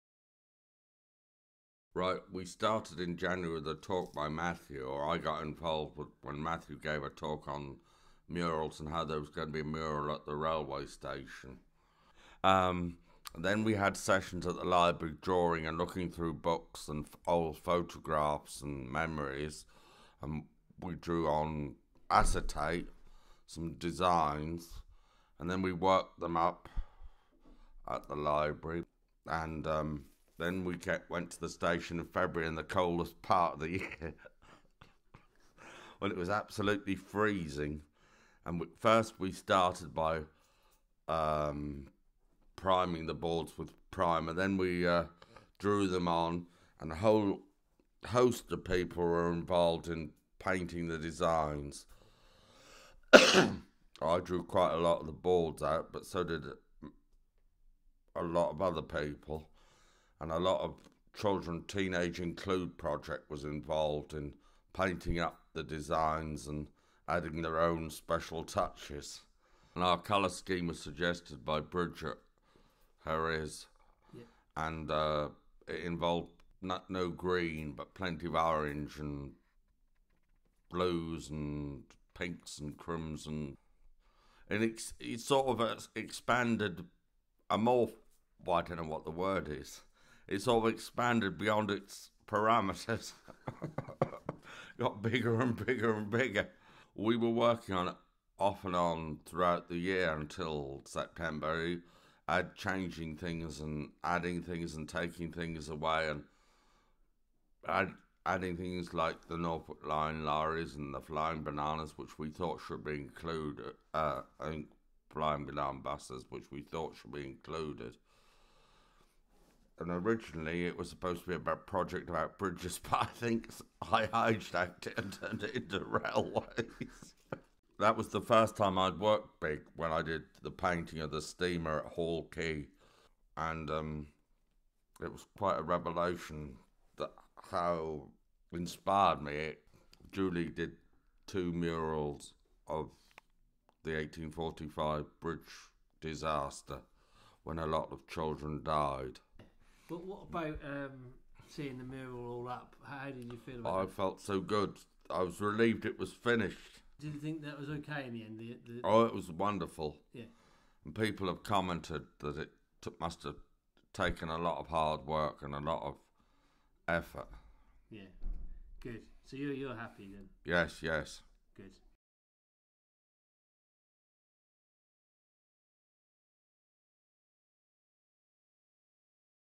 VAUXHALL LINKS COMMUNITY MURAL - MARK LEECH'S MURAL MEMORIES
A short slideshow of photographs documenting the development of the 60 metre long Vauxhall Links Community Mural in Great Yarmouth in between Vauxhall Bridge and Vauxhall Railway Station. The slideshow is voiced by Mark Leech a local artist who helped with the creation of the mural from start to finish. The community mural project was co-ordinated by Matthew Harrison and involved over 60 local people. It was supported by Fair Share, Vauxhall Links and The Heritage Lottery Fund with assistance from Greater Anglia, Linbrook services and Community Connections.